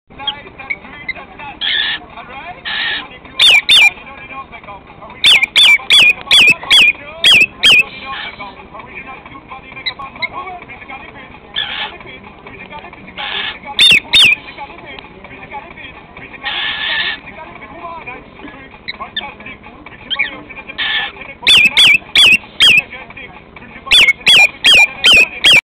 My bird hates this song...
Nala seems to have no problem telling me what songs he dislikes and likes.